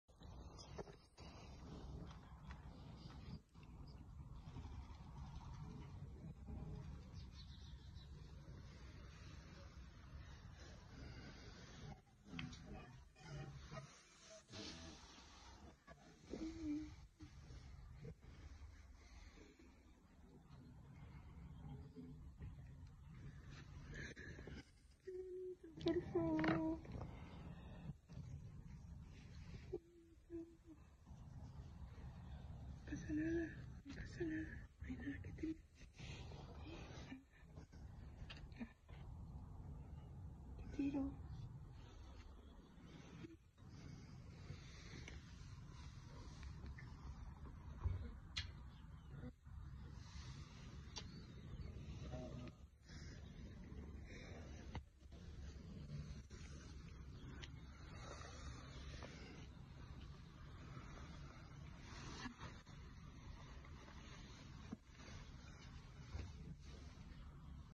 Agnes, una cabrita rescatada enferma, empieza a apreciar el cariño
Agnes ha empezado a perder miedo
Agnes es tan dulce. El primer día temblaba y temblaba cada vez que la tocábamos. Ayer por la noche tras darla el tratamiento tuvo el primer gesto de amor: olió a Laura como sabiendo que puede permitirse ser ella misma y reconocer a las personas que la cuidan y la quieren.
Luego apoyo su cabeza para sentir un beso. El primero en su vida.
Los animales, todos, son sensibles y entienden de emociones incluso más que nosotros.